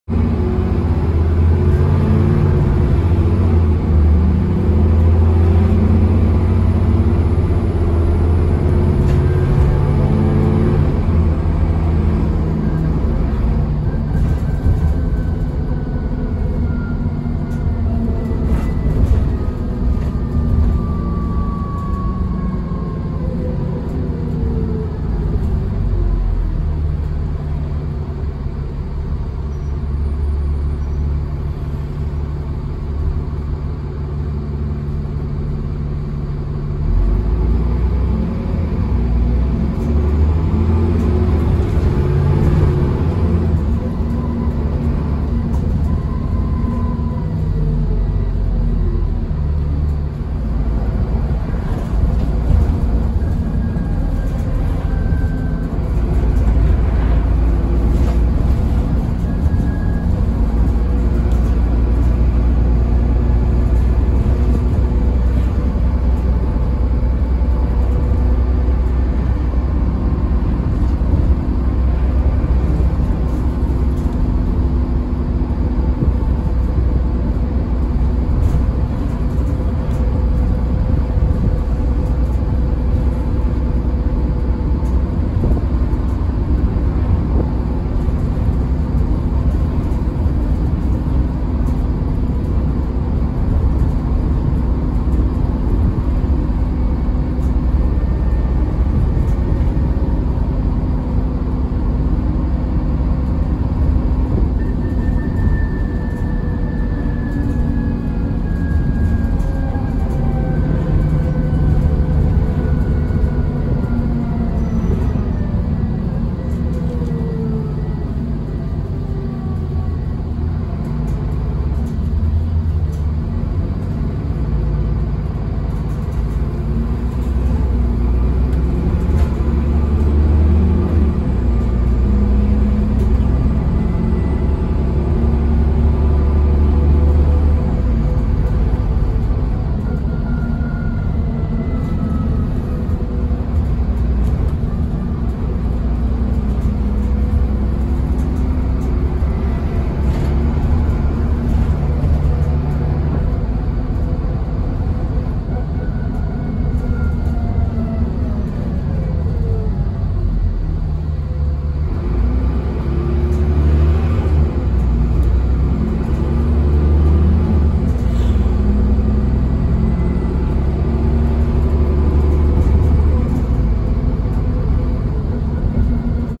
National Express West Midlands 4460 BJ03 EVM Transbus ALX400/Transbus Trident 2 Route WA6
Here is 4460 operating the 6 from Sutton Coldfield to Walsall and filmed between Sutton Coldfield College and Mere Green.